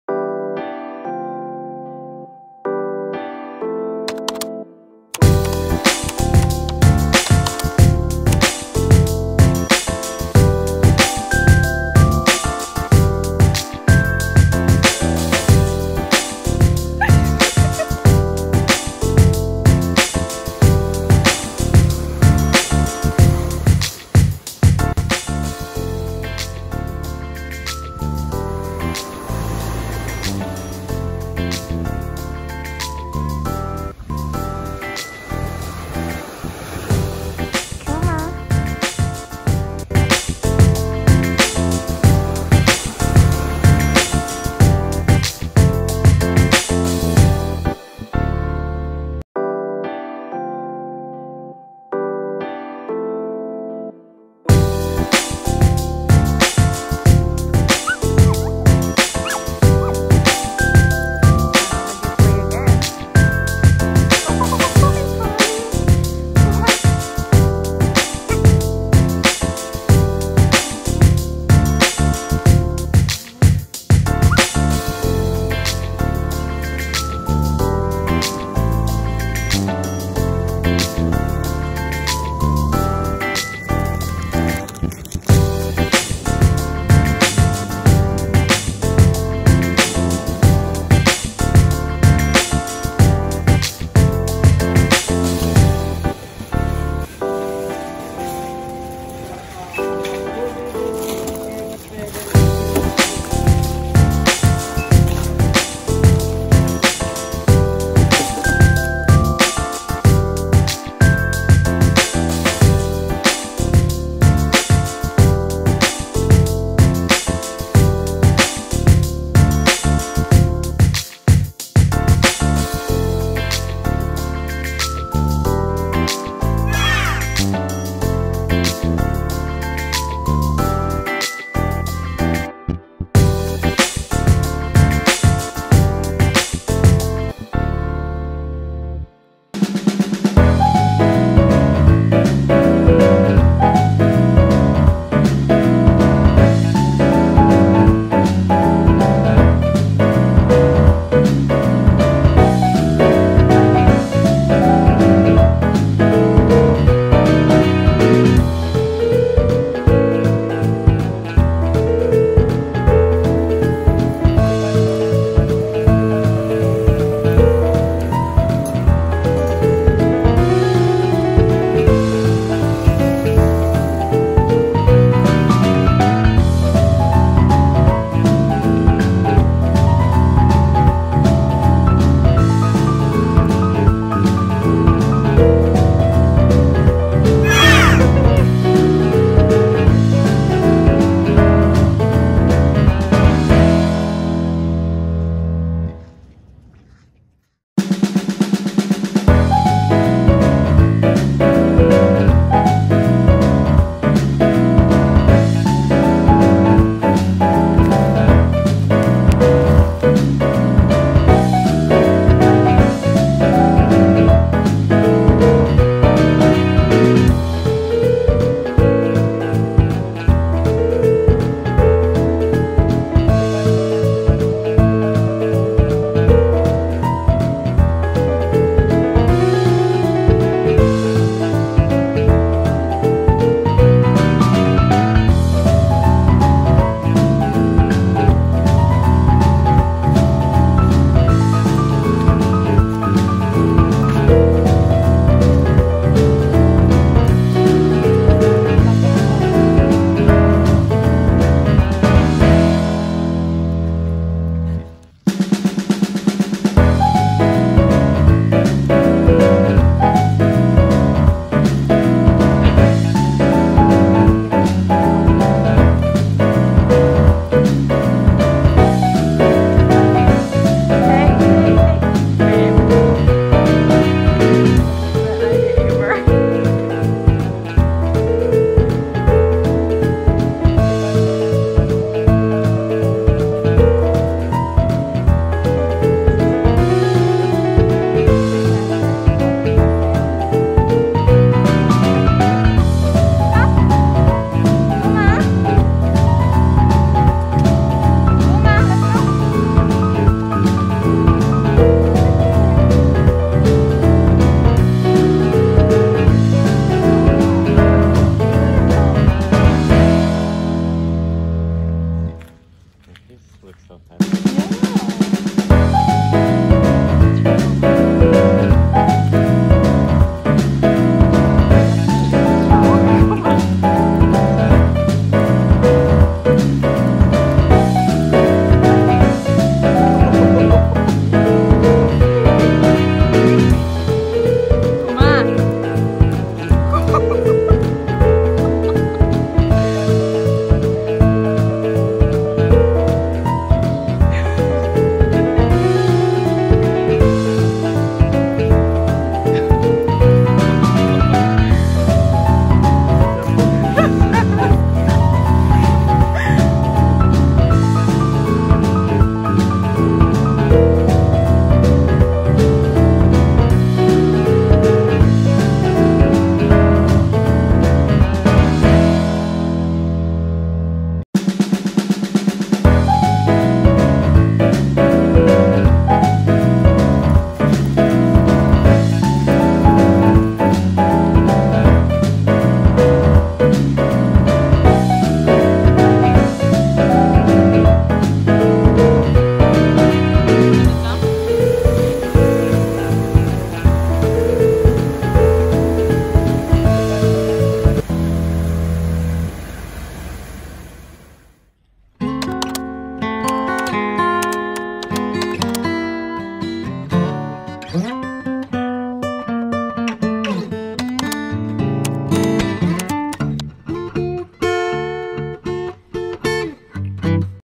Penang Vlog | Seaside | Shiba Inu | Surfskate | A day in my life
Lockdown, a park behind our condo is closed. So, we are trying to find a new spot that has less people for Kuma to be free. Finally, we found the beach, just next to our place and unexpected thing happened 😅